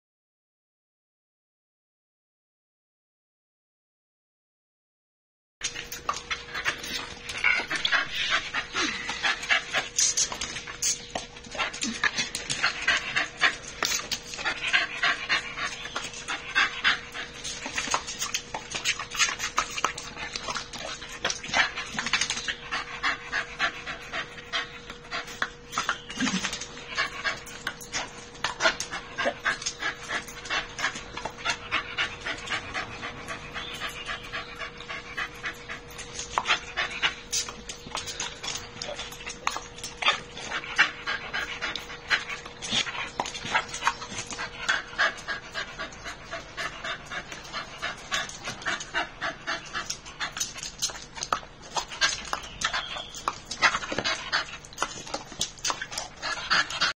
Miniture Bull Terrier ☆ Catch The Water !
She is Qoo of a miniture bull terrier,and 4 years old.
She going to catch water hard.